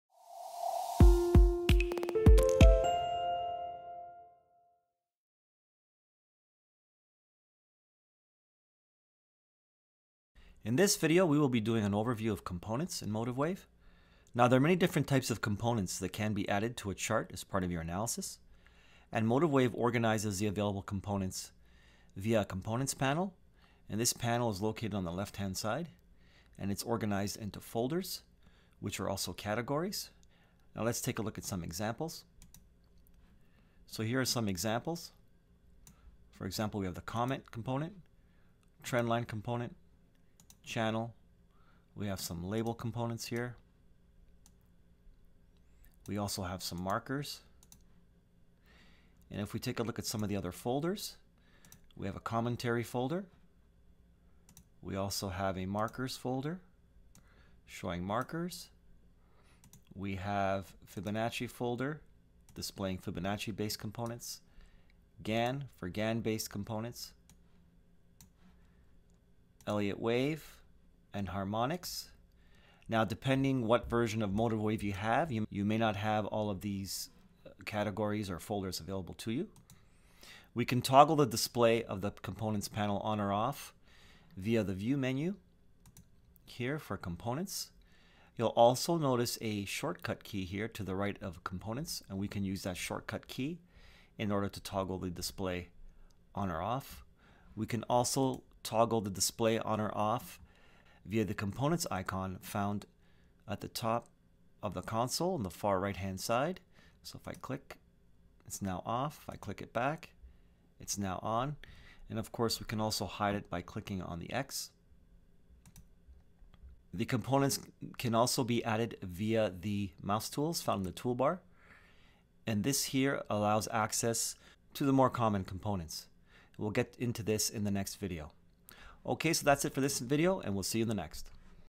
Components Overview in MotiveWave
Overview on Components, the Components Panel and the Components in the Tools Dropdown menu in the MotiveWave trading software.